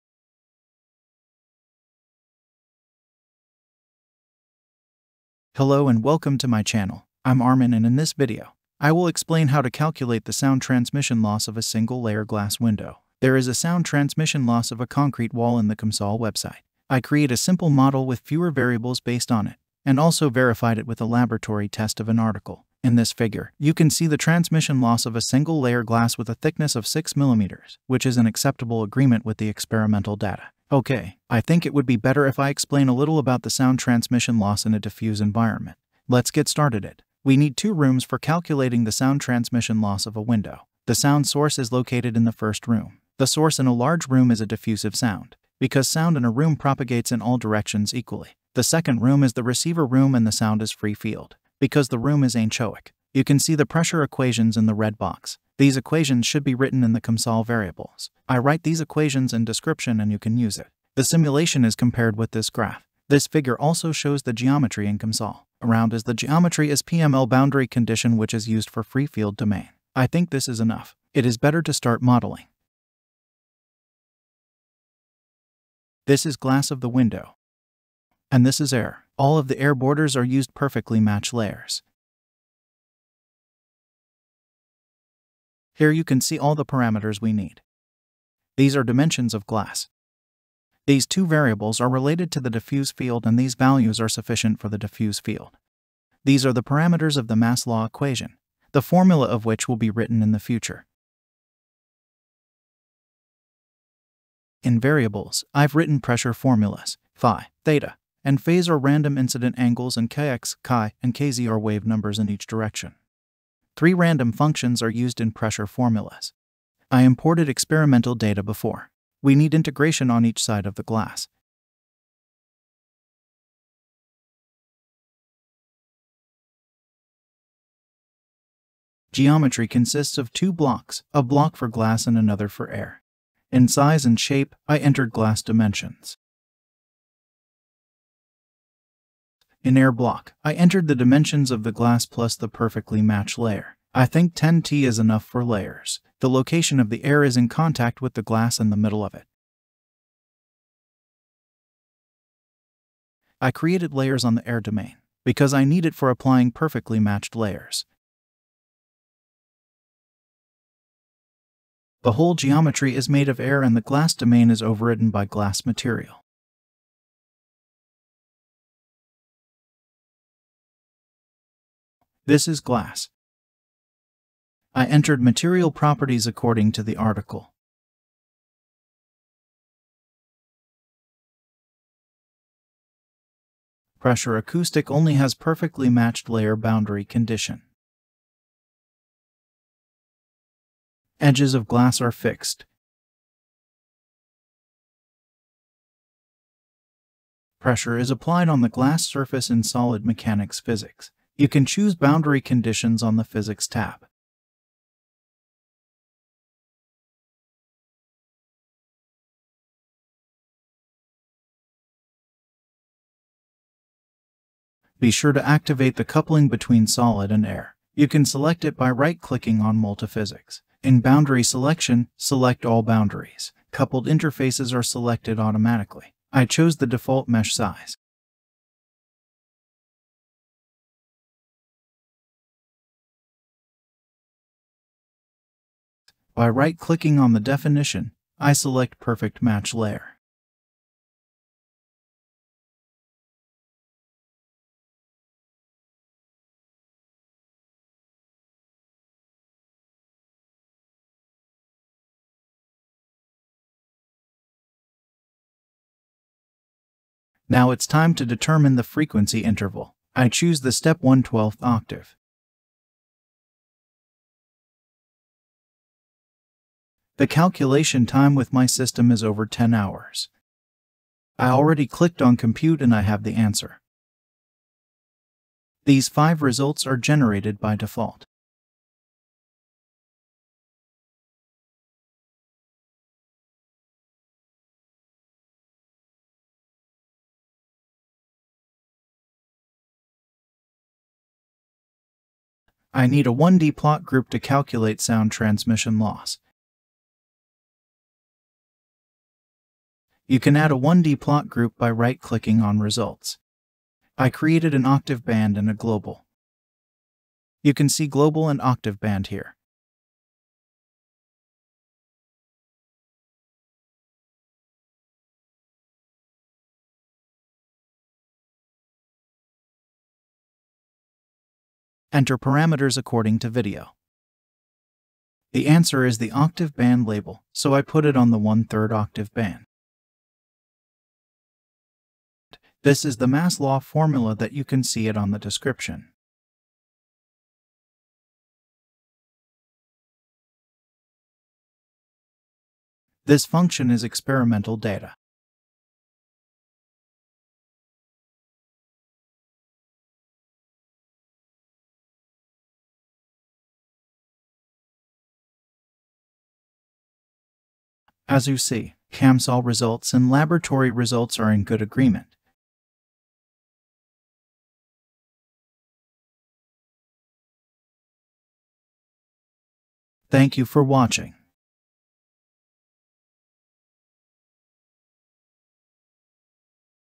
Mastering COMSOL: Sound transmission loss of a panel in COMSOL multiphysics
Comsol multiphysics tutorial
In this video. I will explain how to calculate sound transmission loss of a panel and I validate with experimental data in COMSOL.

prarameters (copy to text file):
T 6[mm] 0.006 m Thickness of concrete wall
H 1.5[m] 1.5 m Height of concrete wall
W 1.25[m] 1.25 m Width of concrete wall
seed 42 42  Seed for random function
N 100 100  Number of random waves in sum
c0 343.2[m/s] 343.2 m/s Speed of sound in air
rho0 1.2[kg/m^3] 1.2 kg/m³ Density of air
fmax 2000[Hz] 2000 Hz Maximum study frequency
m 15[kg/m^2] 15 kg/m² Wall surface density

variables:
phi phi_rnd(n,freq[1/Hz],1,seed)
theta acos(costheta_rnd(n,freq[1/Hz],2,seed))
phase phase_rnd(n,freq[1/Hz],3,seed)
kx cos(theta)*k0  Partial wave vector, x-component
ky sin(theta)*cos(phi)*k0  Partial wave vector, y-component
kz sin(theta)*sin(phi)*k0  Partial wave vector, z-component
k0 2*pi*freq/c0 1/m Propagation constant
p_room 1[Pa]/sqrt(N)*sum(exp(i*phase)*exp(-i*(kx*x+ky*y+kz*z)),n,1,N) Pa Room diffuse field
p_refl 1[Pa]/sqrt(N)*sum(exp(i*phase)*exp(-i*(-kx*x+ky*y+kz*z)),n,1,N) Pa Reflected acoustic field at x=0
p_wall p_room+p_refl Pa Total wall pressure (mechanical load)

comsol simulation
Comsol Sound transmission loss